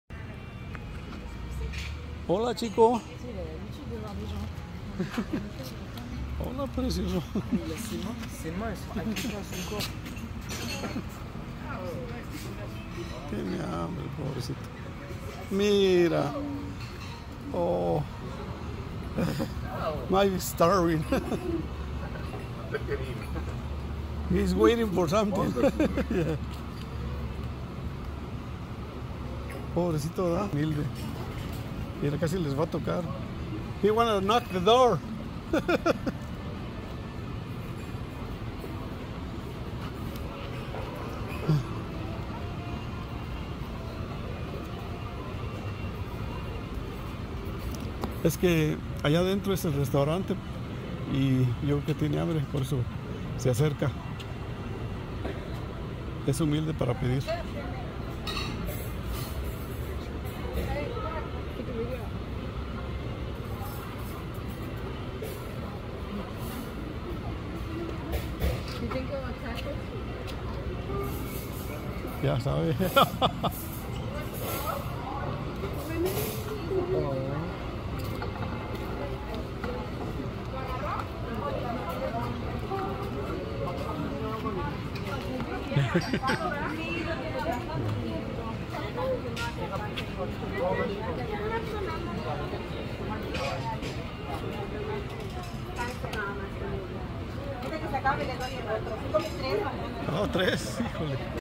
Un mono lindo!!
Un mono qué nos robo el corazón! tiene hambre y sabe cómo manifestarse para atraer la atención a la salida de un restaurante.